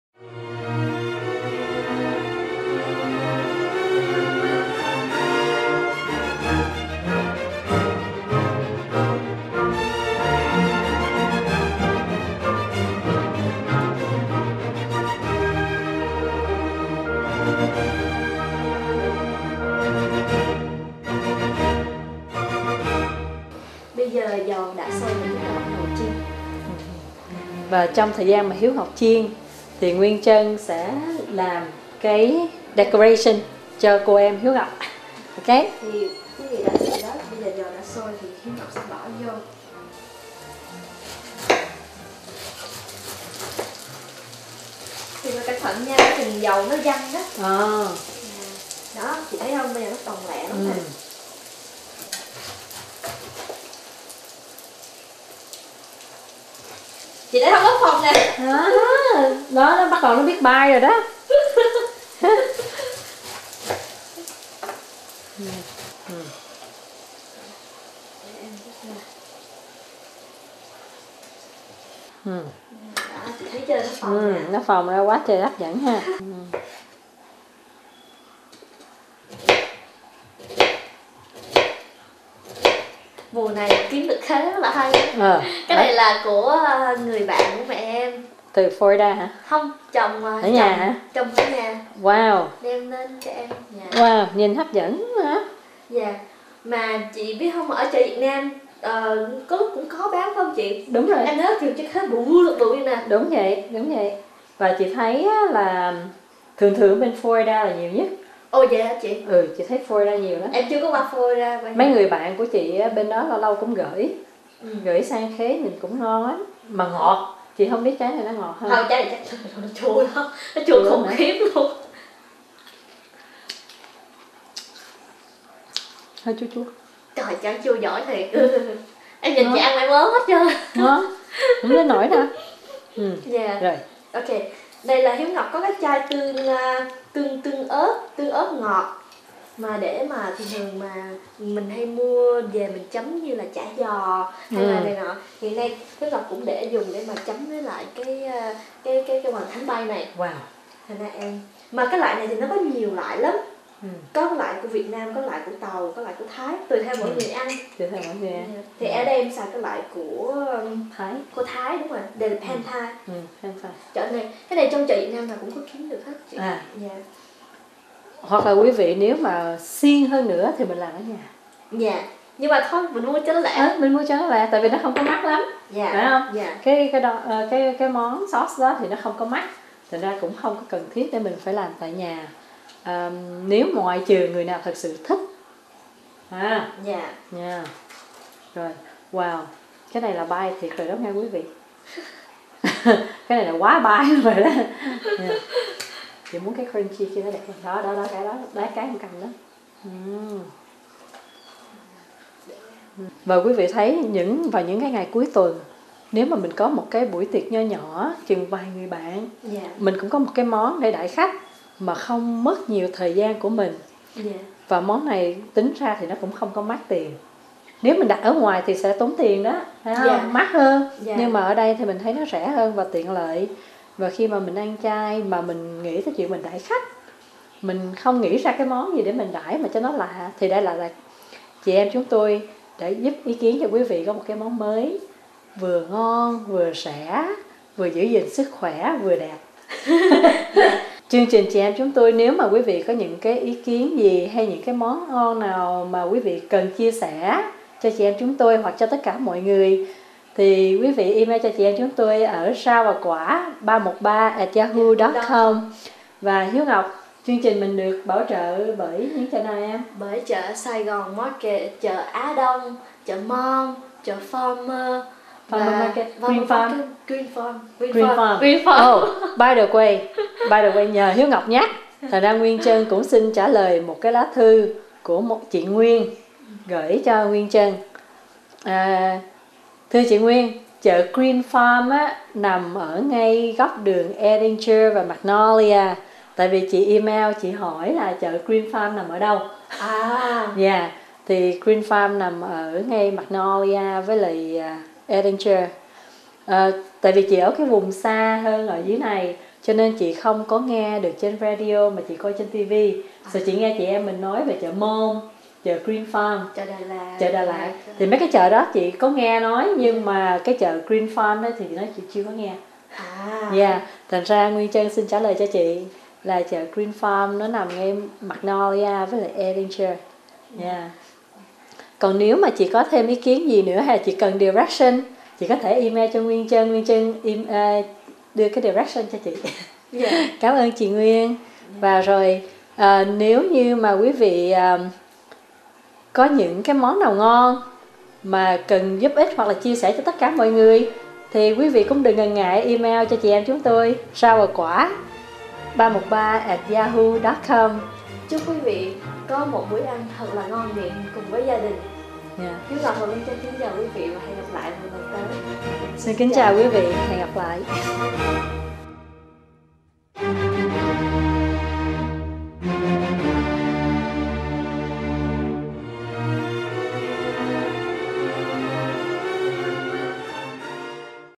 Rau Va Qua (Veggies and Fruits) - Hoành Thánh Bay (Flying Wonton) part2
Fried Stuffed wonton with mushrooms, onion, leeks, cream cheese, and soy paste